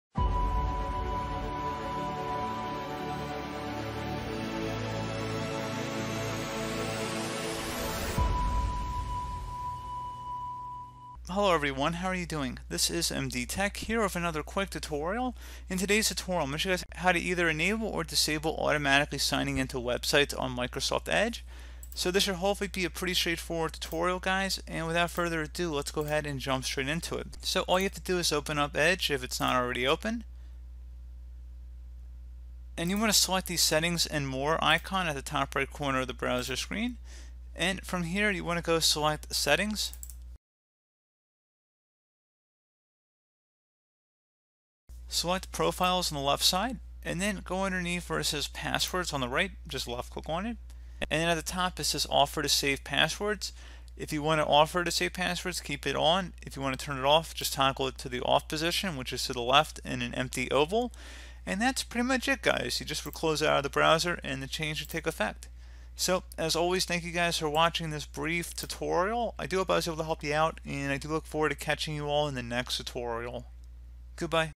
How to Turn off Google Chrome Auto Sign-In!
Issues addressed in this tutorial:
chrome auto sign in not working
google chrome auto sign out
auto sign in google chrome
auto sign out gmail chrome

Previously when using the Chrome Browser, users could sign into various Google websites like Gmail, Google Docs, or Google Drive without needing to sign into the Chrome browser itself.